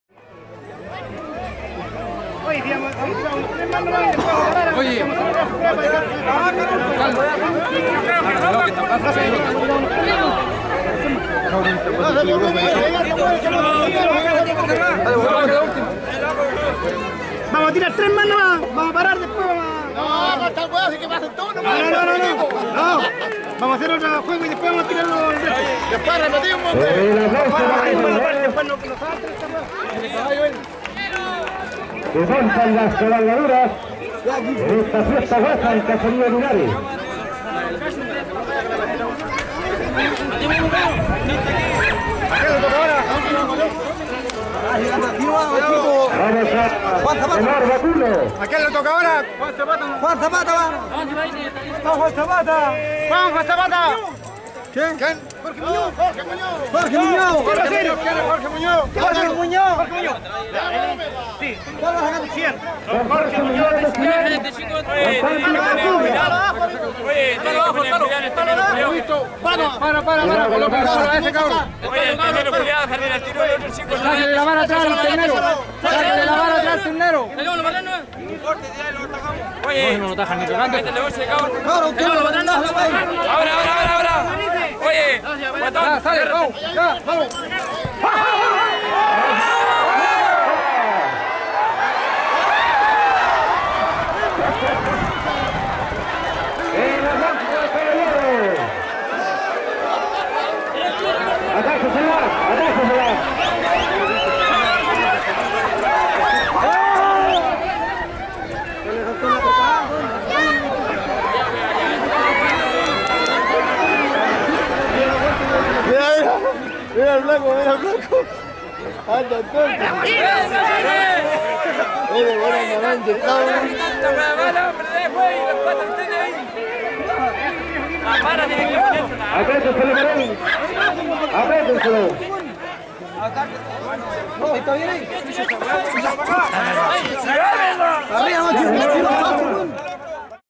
Fiesta Huasa Caserio Linares - Domaduras de Novillo I
En la Fiesta Huasa del Sector de Caserio Linares Comuna de Chillán año 2011